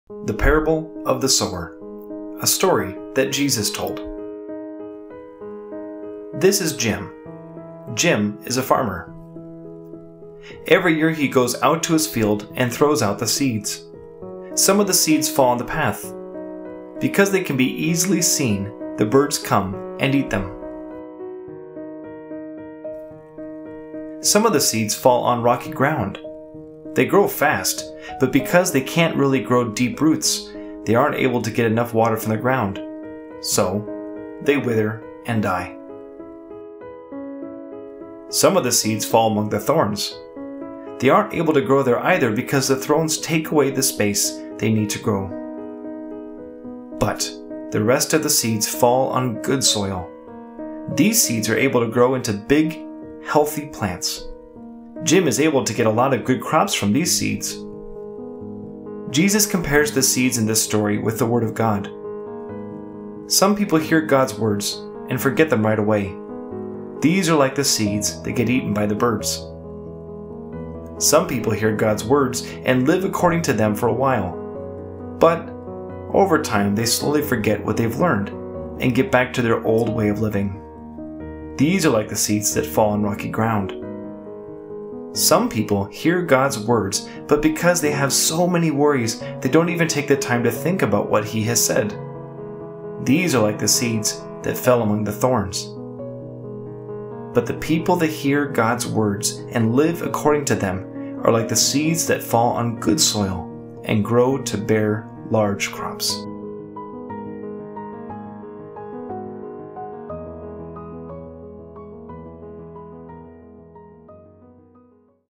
The Parable of the Sower
Luke 8:5-15
5 “The farmer went out to sow his seed. As he sowed, some fell along the road, and it was trampled under foot, and the birds of the sky devoured it. 6 Other seed fell on the rock, and as soon as it grew, it withered away, because it had no moisture. 7 Other fell amid the thorns, and the thorns grew with it, and choked it. 8 Other fell into the good ground, and grew, and produced one hundred times as much fruit.” As he said these things, he called out, “He who has ears to hear, let him hear!” 9 Then his disciples asked him, “What does this parable mean?” 10 He said, “To you it is given to know the mysteries of God’s Kingdom, but to the rest in parables; that ‘seeing they may not see, and hearing they may not understand.’ Now the parable is this: The seed is the word of God. 12 Those along the road are those who hear, then the devil comes, and takes away the word from their heart, that they may not believe and be saved. 13 Those on the rock are they who, when they hear, receive the word with joy; but these have no root, who believe for a while, then fall away in time of temptation. 14 That which fell among the thorns, these are those who have heard, and as they go on their way they are choked with cares, riches, and pleasures of life, and bring no fruit to maturity. 15 Those in the good ground, these are those who with an honest and good heart, having heard the word, hold it tightly, and produce fruit with perseverance.